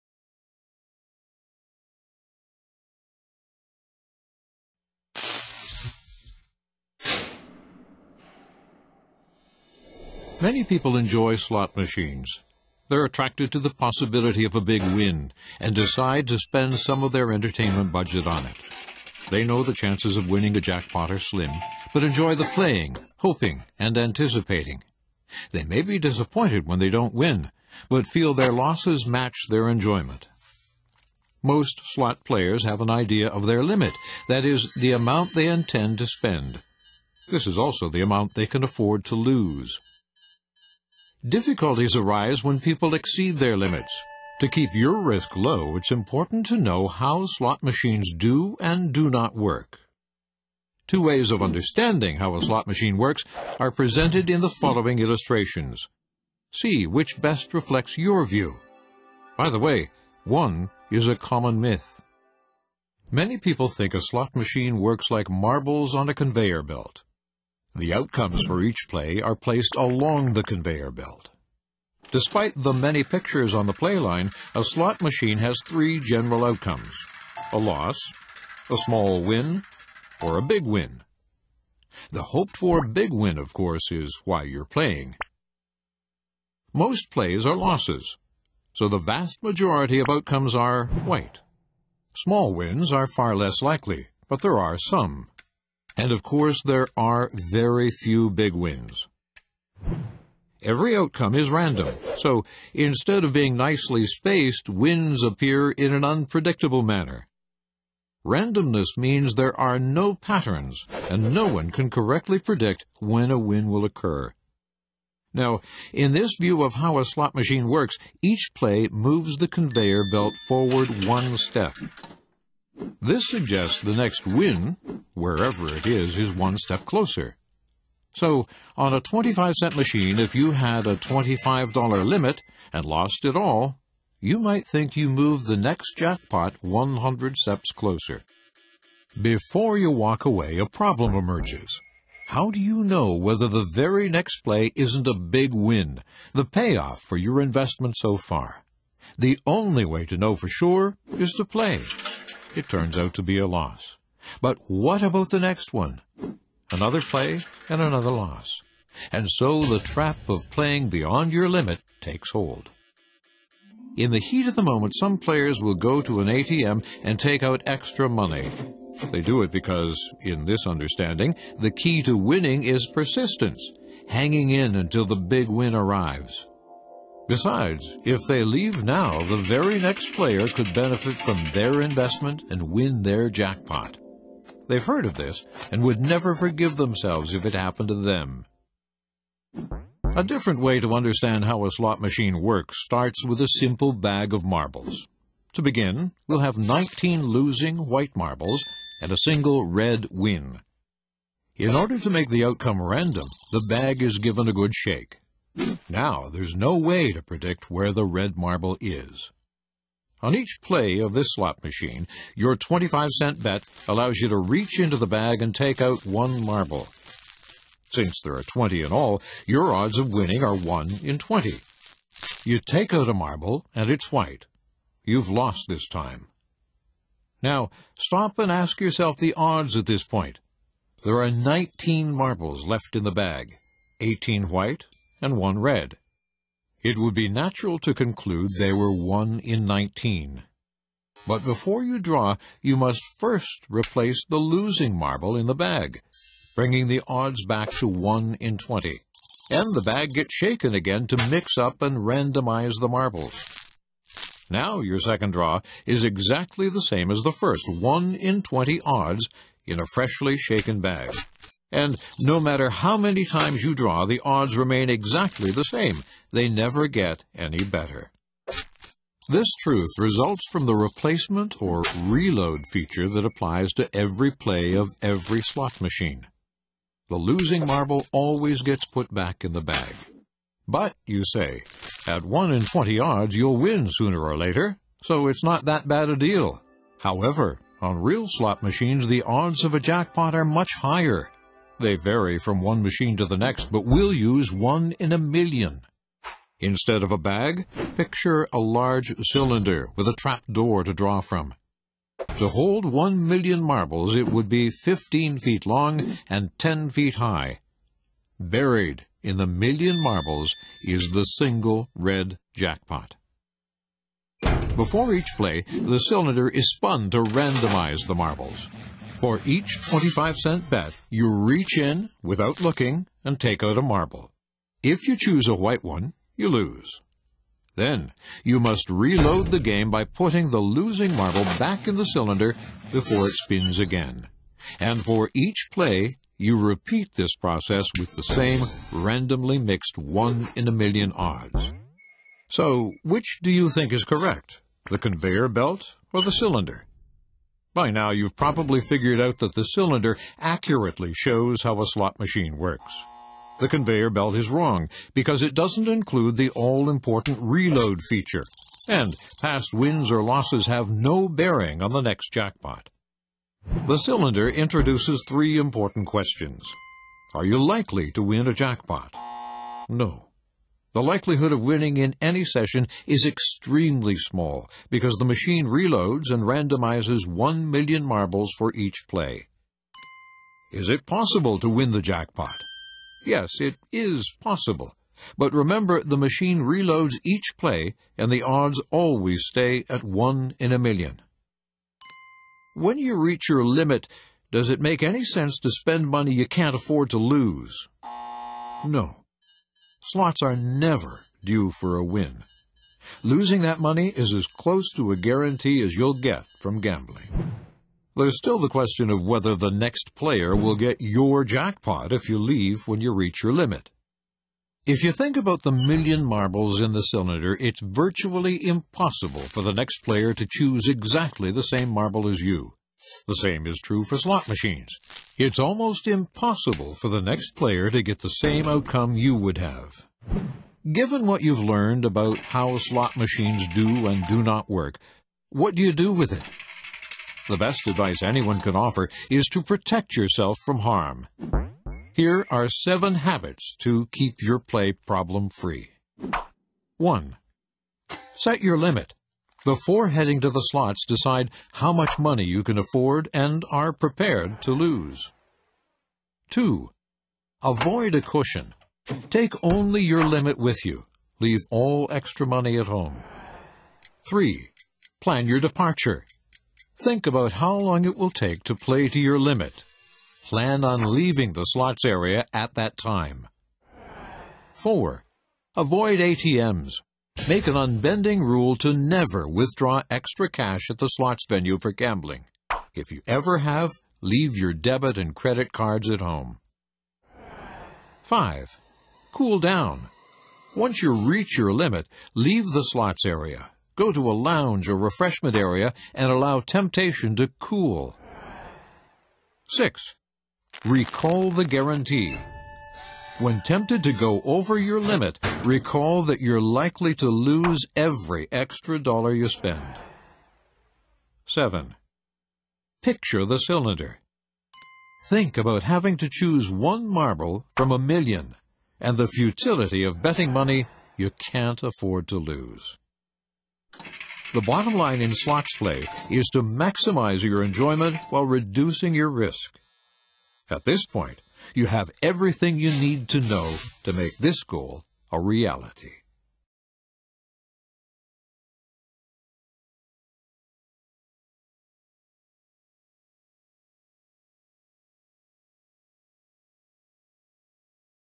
How Slot Machines Work - GameSense
All the lucky charms in the world won't affect the results of games of chance. Every roll, throw or spin has an equal chance of winning or losing. It doesn't matter if you just lost three times in a row, or 40 times. The odds of winning don't change. The only thing you can control is how much you bet, how often you play, and for how long. That's why many players set a limit beforehand — it's the best way to keep chance-based games fun.

Some examples of games based on chance include slot machines, baccarat, craps, roulette, keno, lottery, scratch tickets, and bingo.